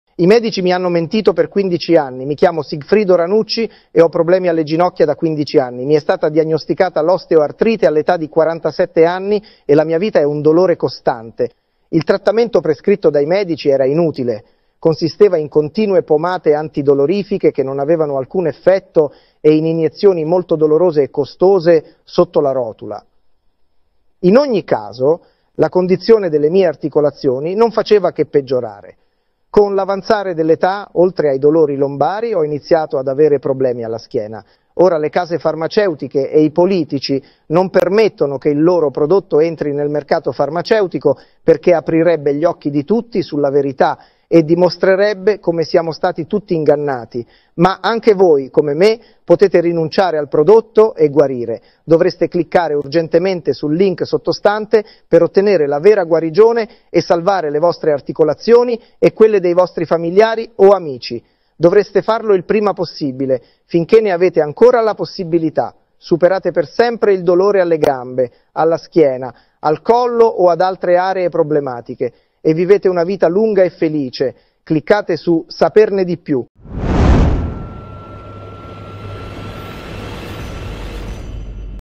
Deepfake con Sigfrido Ranucci
Ecco un altro video deepfake. Questo utilizza delle clip di Sigfrido Ranucci a sua insaputa, modificate con l'intelligenza artificiale, per pubblicizzare improbabili medicinali. Ovviamente, il protagonista non ha mai fatto queste dichiarazioni.

👀 Segnalo quotidianamente i deepfake truffaldini più diffusi al fine di aiutare le persone ad evitarli e ad avere consapevolezza sul fenomeno.